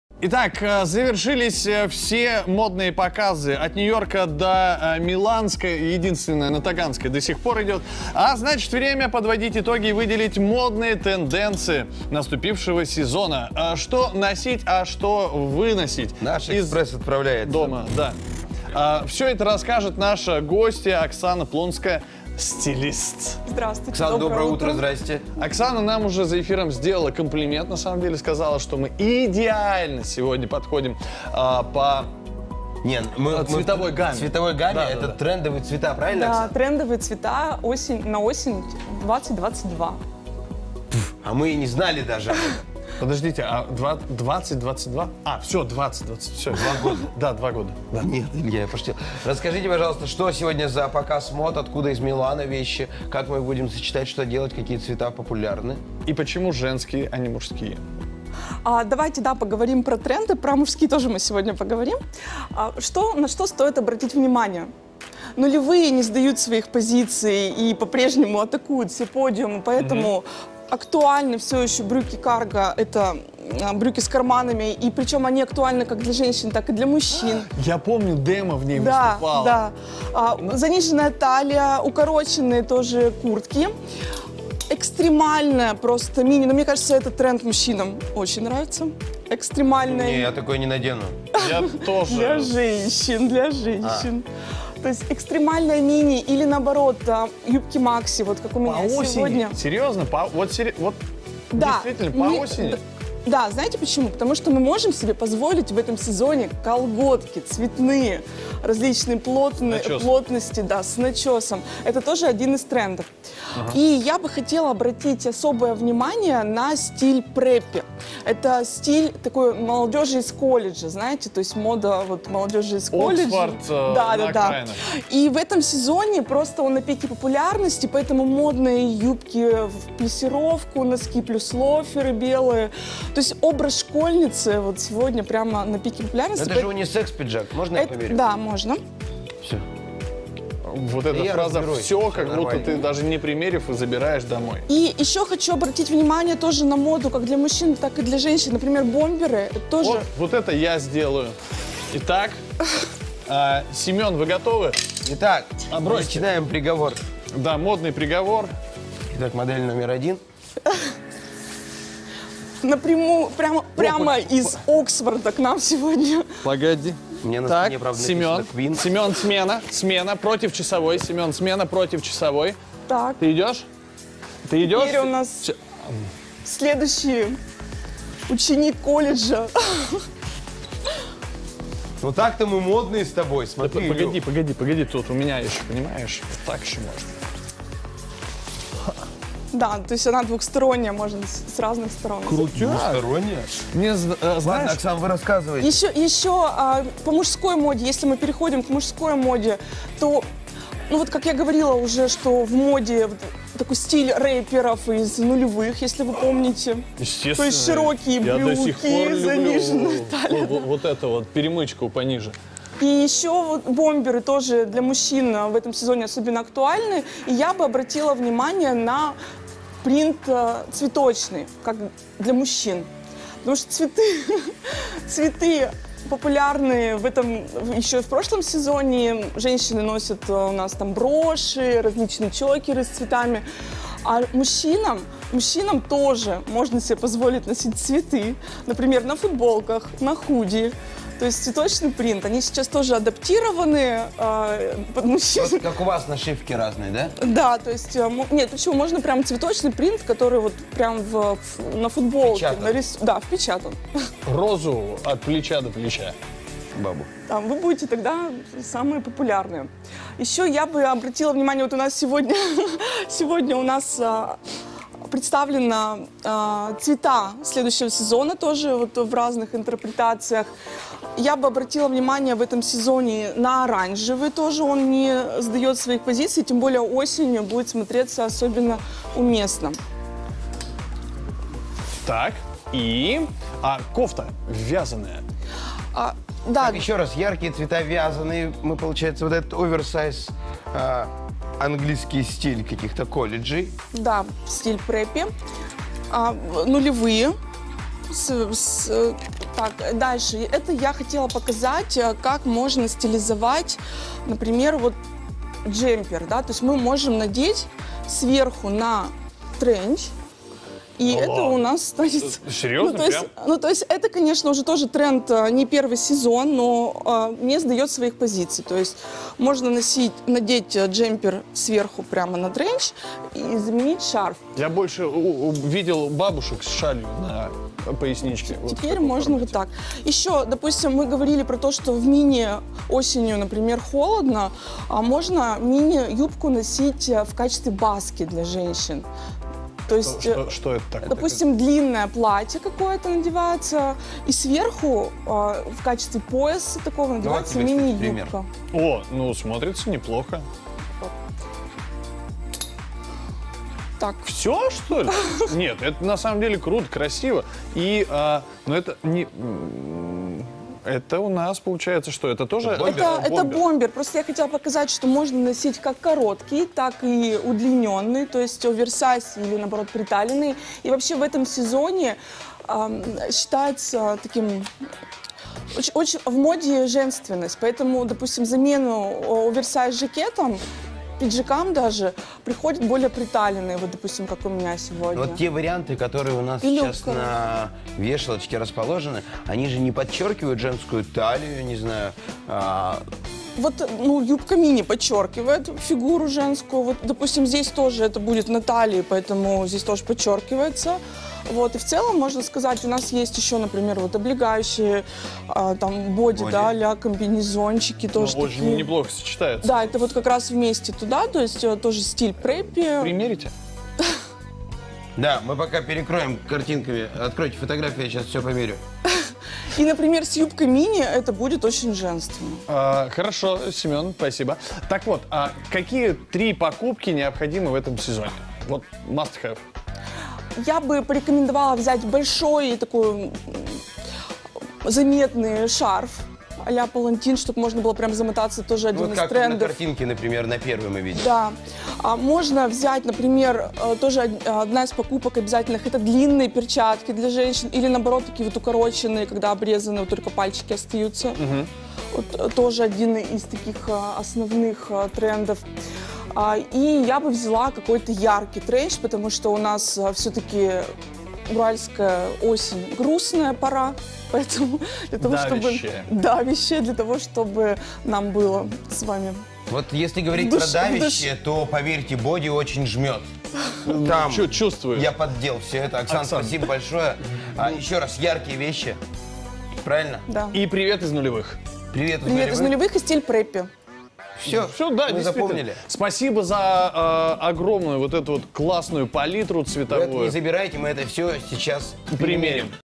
ГЛАВНЫЕ ТРЕНДЫ ОСЕНИ 2022. Фишки для неповторимого образа от Оксаны Плонской 22.09.22
На стиле, на расслабоне! Стилист Оксана Плонская знает, как добавить красок в тоскливую осень. Разбираем основные тренды холодного сезона и стильные "фишки", которые сделают ваш образ неповторимым!